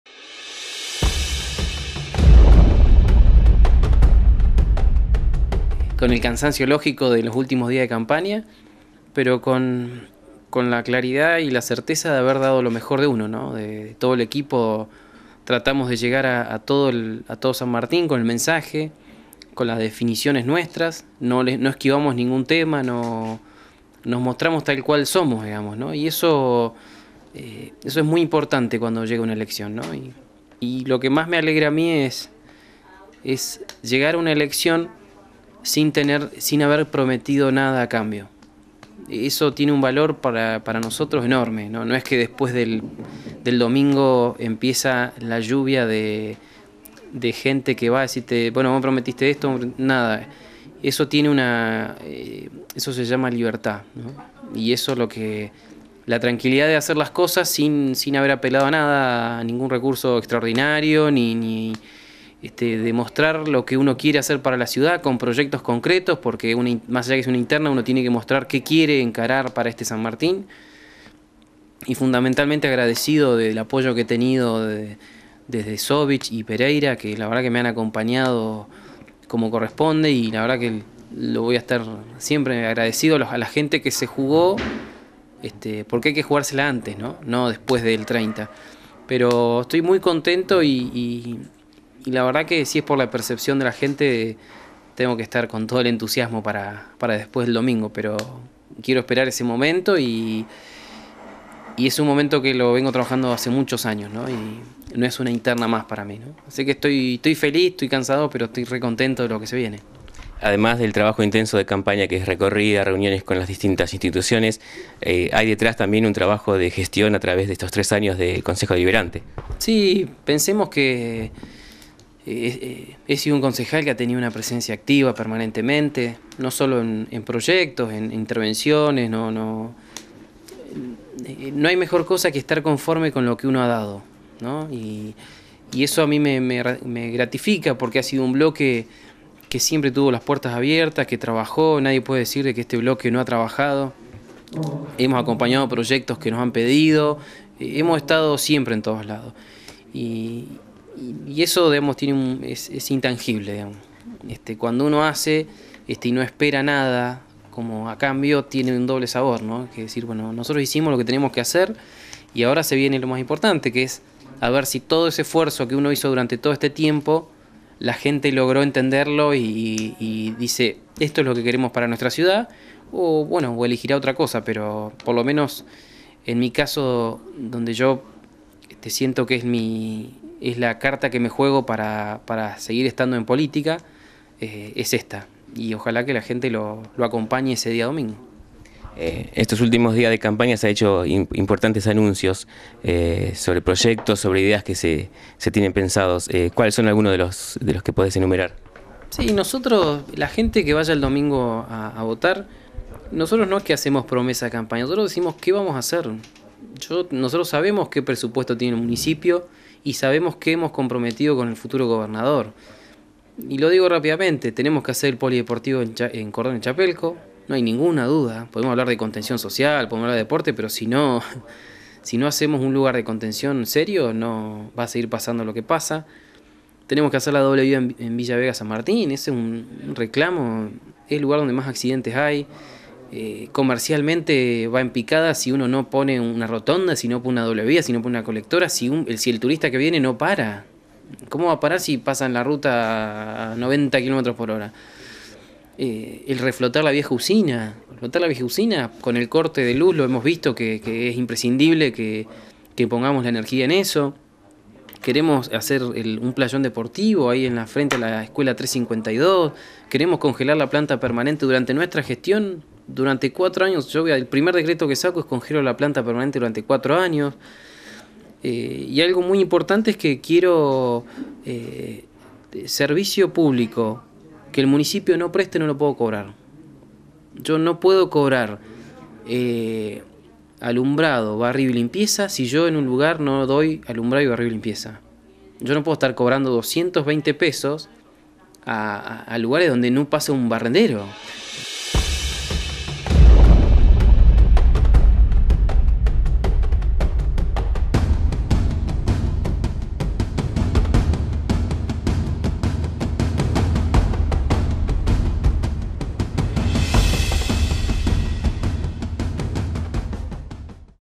Carlos Saloniti: "lo que más me alegra a mi, es llegar a una..."
Carlos Saloniti: "lo que más me alegra a mi, es llegar a una elección sin haber prometido nada a cambio"
A pocas horas de las internas del MPN, hablamos con Carlos Saloniti, precandidato a Intendente por la Lista Azul y Blanca. Cansado pero feliz por el trabajo que vienen realizando en sus recorridas por los barrios, o reuniones con diferentes instituciones, Saloniti agradeció a Pereyra y Sobisch por el apoyo recibido. Un valor agregado al momento de una elección, es la gestión y Saloniti forma parte del bloque de concejales junto a Natalia Vita y Daniel Troncoso, "he sido un Concejal que ha tenido una presencia activa permanentemente, no solo en proyectos, intervenciones (...) ha sido un bloque que siempre tuvo las puertas abiertas, que trabajó...", señaló.
Entre los anuncios que vienen haciendo está el polideportivo en Cordones de Chapelco, la doble vía en Villa Vega San Martín y una rotonda a la altura de la YPF, reflotar la vieja usina, un playón deportivo frente a la Escuela 352 y congelar la planta permanente durante la gestión.-